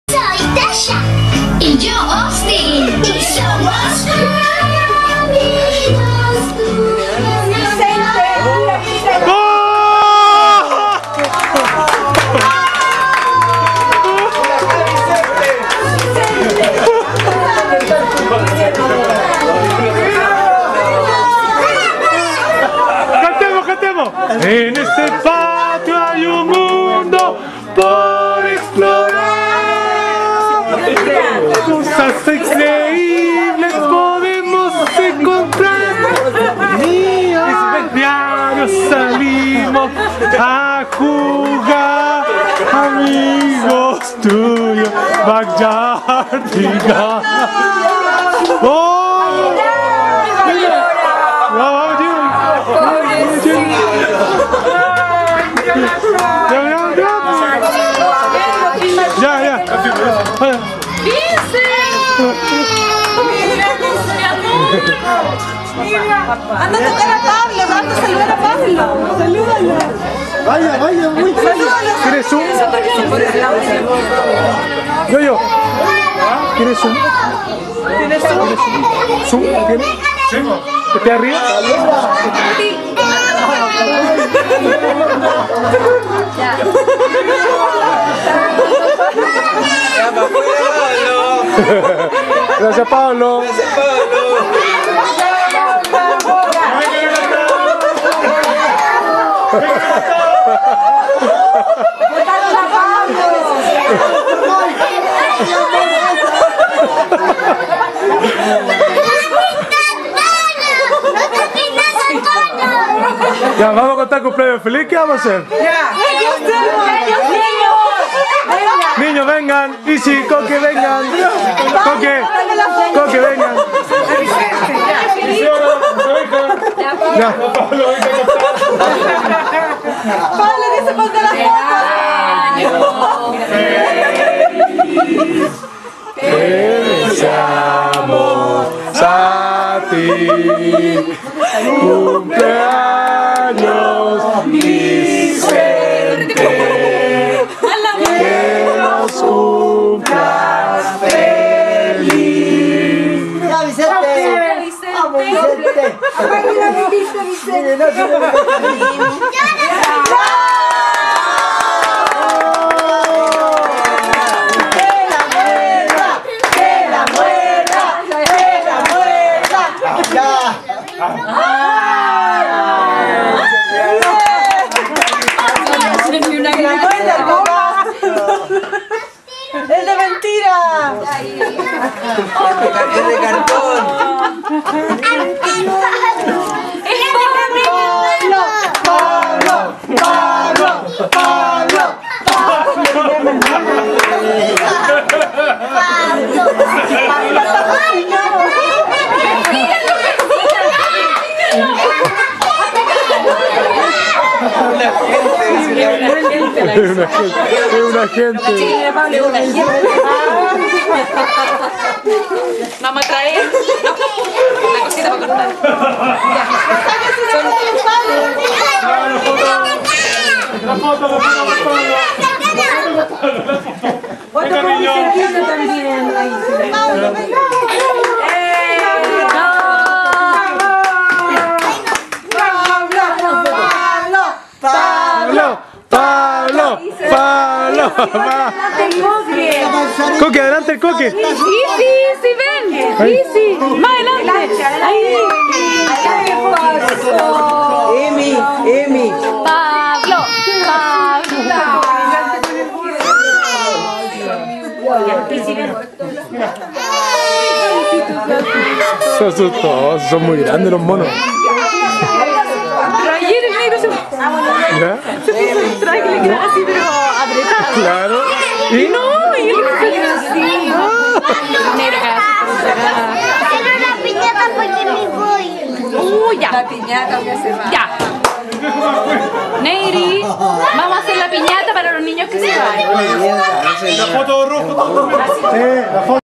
KOKOCHO celebra su CUMPLEAÑOS Nª1
Con la asistencia de Pablo de los Backtardigans, celebramos el cumpleaños de nuestro hijo VICENTE ("KOKOCHO"). HIJO, QUE SEAS MUY FELIZ!!!